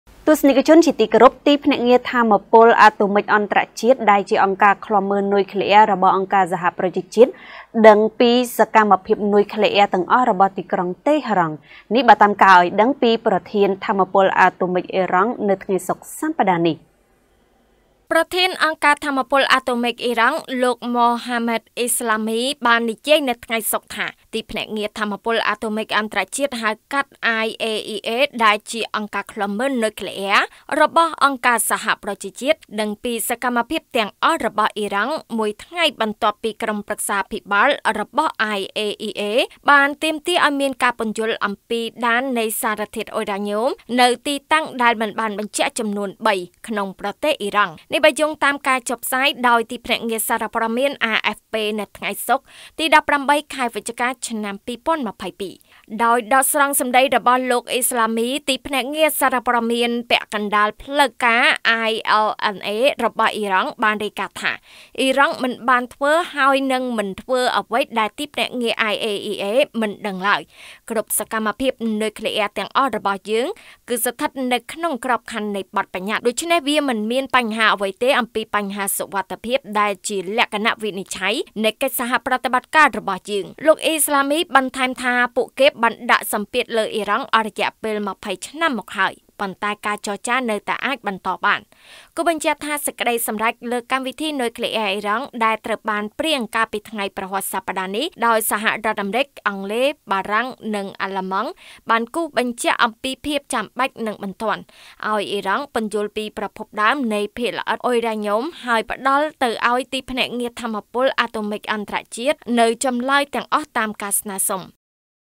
ប្រធានអង្គការថាមពលអាតូមិកអ៉ីរ៉ង់ ថា IAEA ដឹងពីសកម្មភាពនុយក្លេអ៊ែរទាំងអស់ របស់ទីក្រុងតេហេរ៉ង់
* រាយការណ៍ ដោយកញ្ញា  យឹម ចាន់ស្រីវីន និងអ្នកនាង កេត សត្យា,